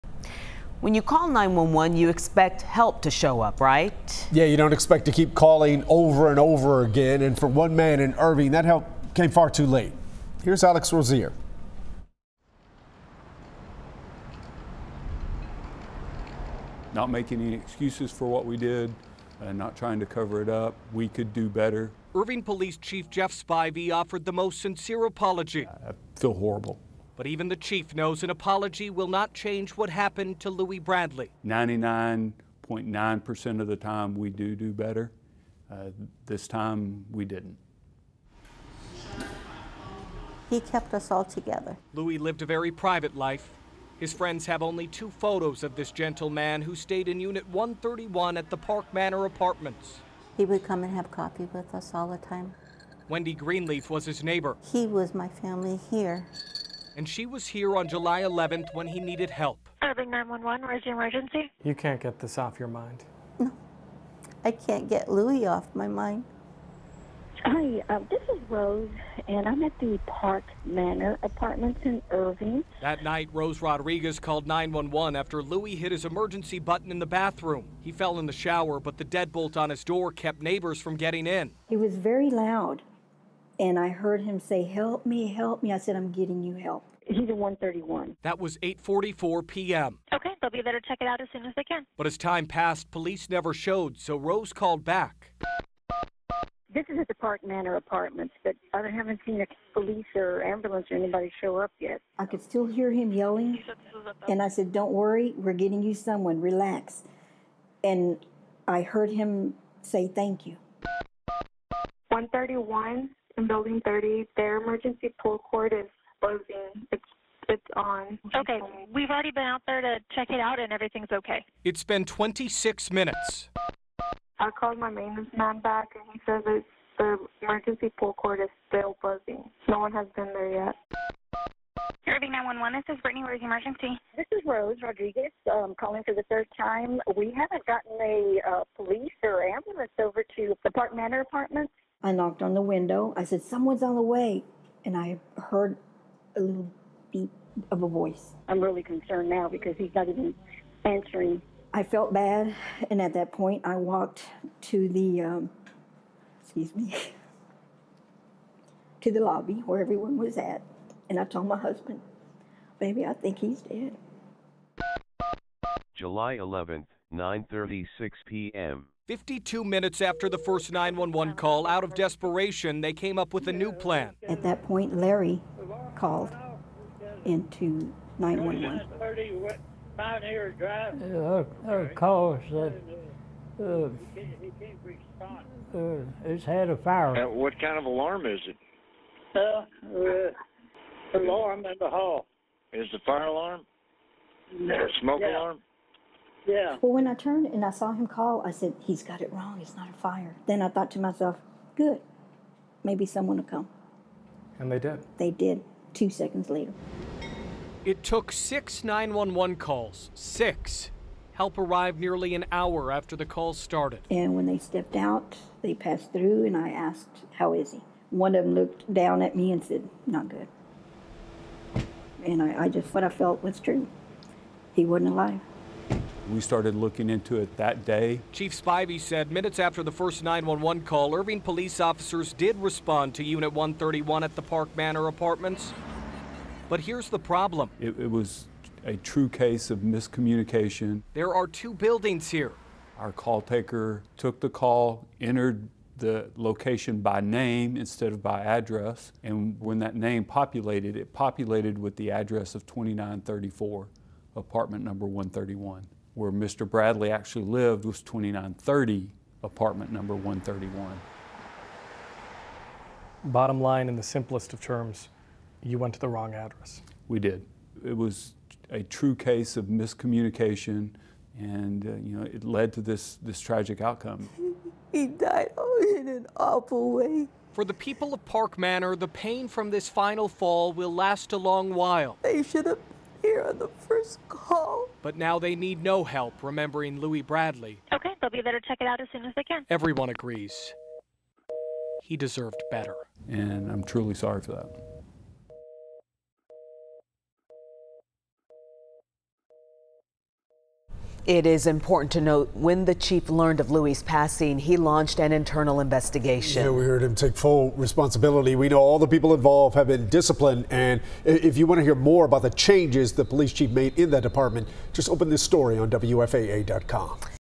Six 911 calls were made after a man fell in his shower. So why did help arrive too late?
Irving police say miscommunication and a strange coincidence led to a response that was too late for Louie Bradley. STORY